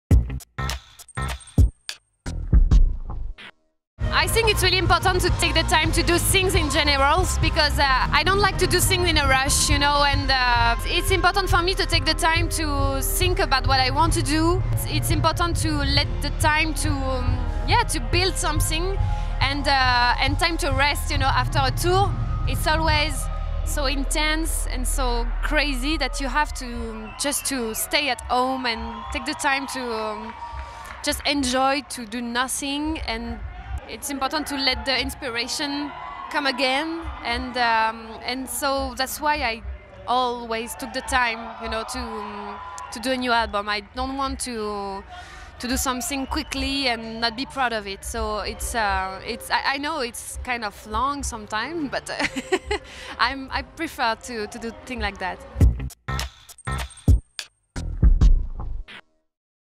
Yelle Talks Creative Process: Breaks From Recording Unintentional, But Necessary
Yelle Addresses Why its Important for the Band to Take Breaks From Recording

AllMusic sat down with French pop singer Yelle to get insight on whether or not the long gaps between albums are intentional.